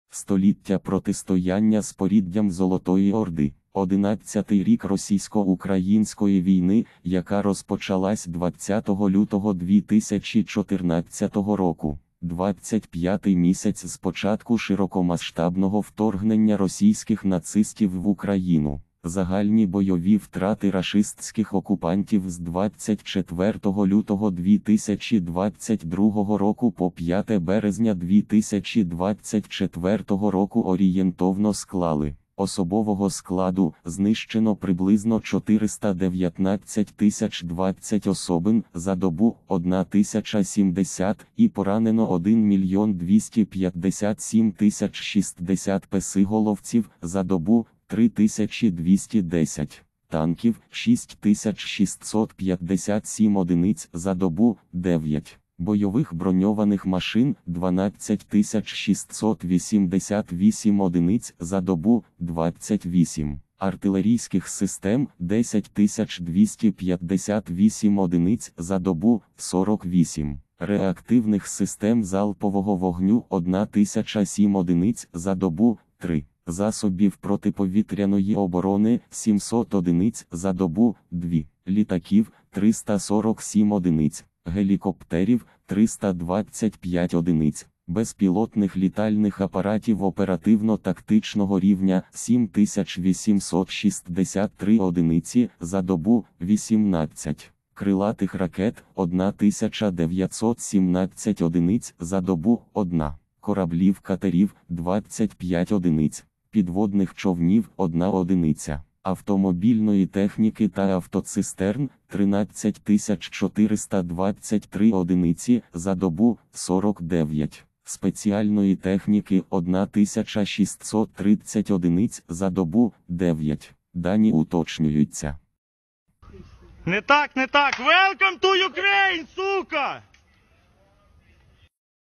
ЗСУ знищили 700 засобів протиповітряної оборони і понад 13400 одиниць автотехніки рашистів
Століття протистояння з поріддям Золотої Орди. Одинадцятий рік російсько-української війни, яка розпочалась 20 лютого 2014 року. Двадцять п'ятий місяць з початку широкомасштабного вторгнення російських нацистів в Україну.

Загальні бойові втрати рашистських окупантів з 24 лютого 2022 року по 05 березня 2024 року орієнтовно склали:
* особового складу — знищено приблизно 419020 особин (за добу – 1070) і поранено 1257060 песиголовців (за добу – 3210);
* танків — 6657 одиниць (за добу – 9);
* бойових броньованих машин — 12688 одиниць (за добу – 28);
* артилерійських систем — 10258 одиниць (за добу – 48);
* реактивних систем залпового вогню — 1007 одиниць (за добу – 3);
* засобів протиповітряної оборони — 700 одиниць (за добу – 2);
* літаків — 347 одиниць;
* гелікоптерів — 325 одиниць;
* безпілотних літальних апаратів оперативно-тактичного рівня — 7863 одиниці (за добу – 18);
* крилатих ракет — 1917 одиниць (за добу – 1);
* кораблів / катерів — 25 одиниць;
* підводних човнів — 1 одиниця;
* автомобільної техніки та автоцистерн — 13423 одиниці (за добу – 49);
* спеціальної техніки — 1630 одиниць (за добу – 9).

Дані уточнюються.

Мінус 1070 окупантів, знищено 28 ББМ та 48 артсистем: втрати армії рф за добу – АрміяInform

Загальні бойові втрати росії з початку війни з Україною

ТОП-10 воєнних злочинів, вчинених армією нелюдів в Україні – АрміяInform

Втрати російської армії під час вторгнення в Україну — Вікіпедія